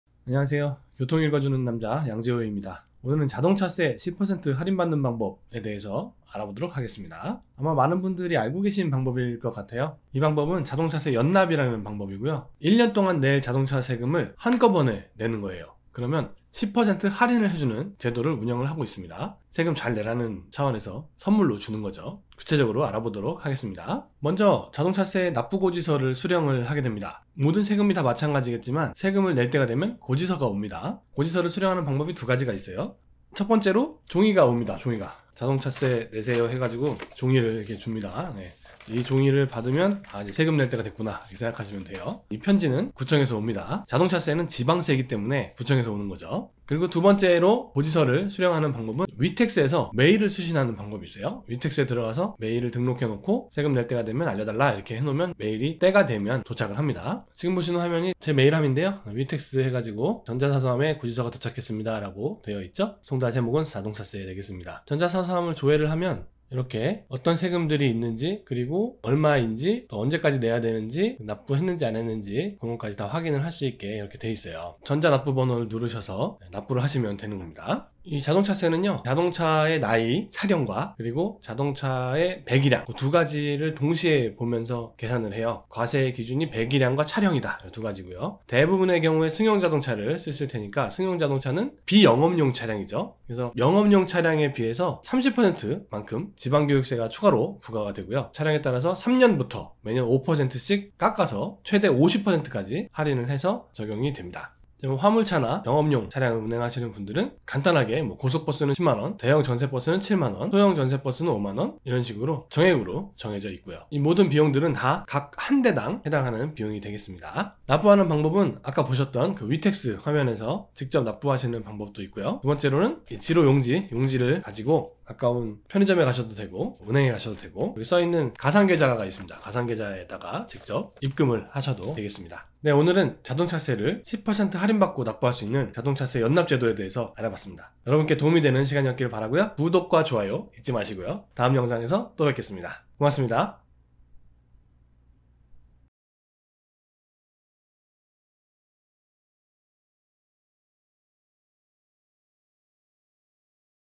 자동차세 10% 할인받는 방법(자동차세 연납제도)
0:00 INTRO
0:10 자동차세 연납제도 소개
0:24 납부고지서 수령방법
1:17 비영업용 차량 자동차세 부과기준
1:44 영업용(화물, 버스) 차량 자동차세 부과기준
1:57 납부방법 1. 위택스(Wetax) 납부
2:02 납부방법 2. 지로납부
2:12 Outro

추가 팁!
1. 상품권을 사서 포인트 전환 후 납부하시면 조금 더 싸게 내실 수 있습니다.
2. 한번 연납을 신청하시면 다음해부터는 자동으로 연납신청된 고지서가 발부됩니다.